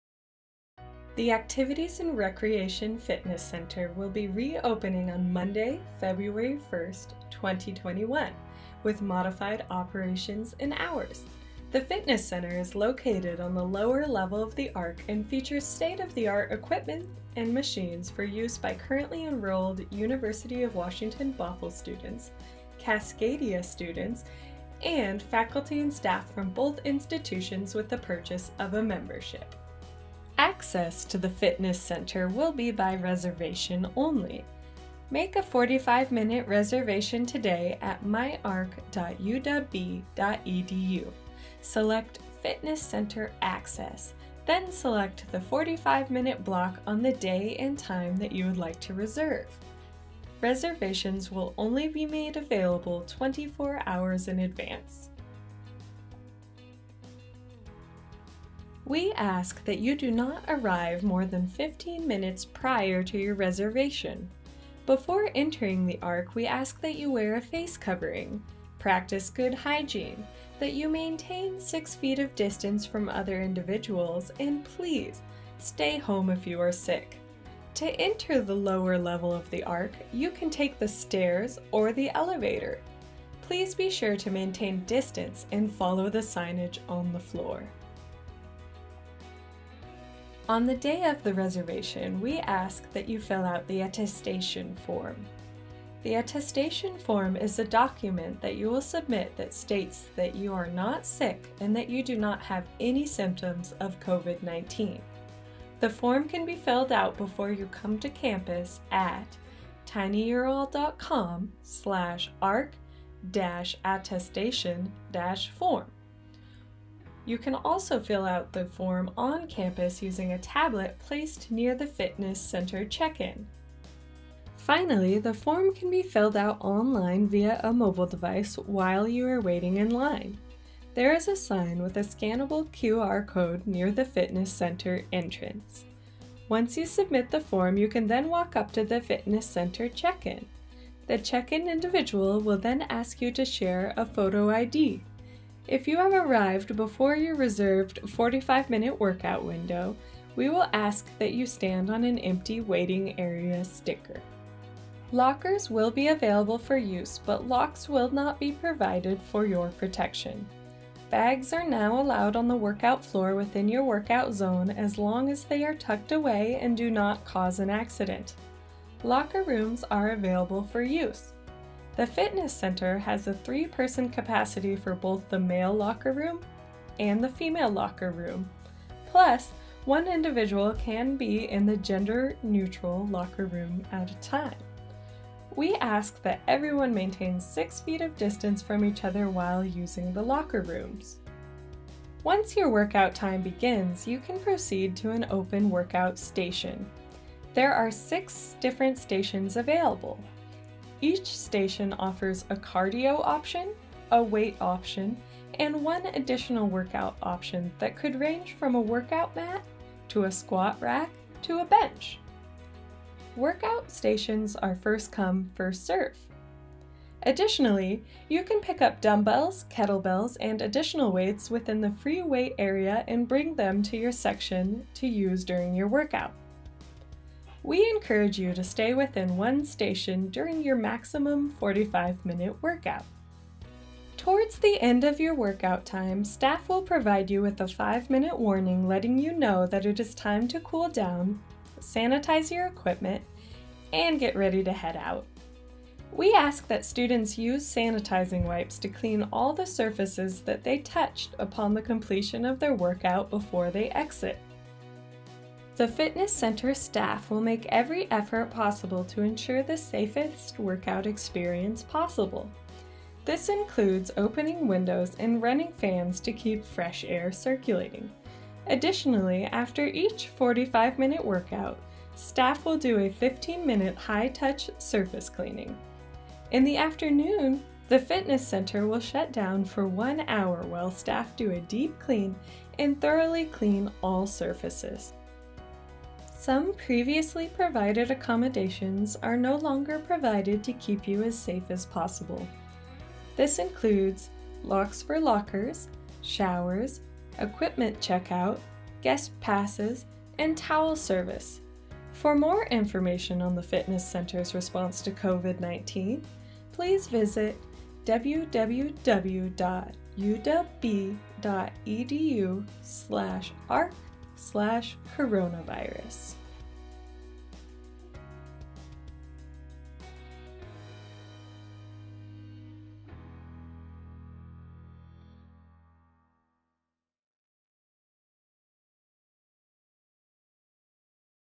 Activities & Recreation Center - Fitness Center Reopening Video
The Activities and Recreation Fitness Center will be reopening on Monday, February 1st, 2021 with modified operations and hours.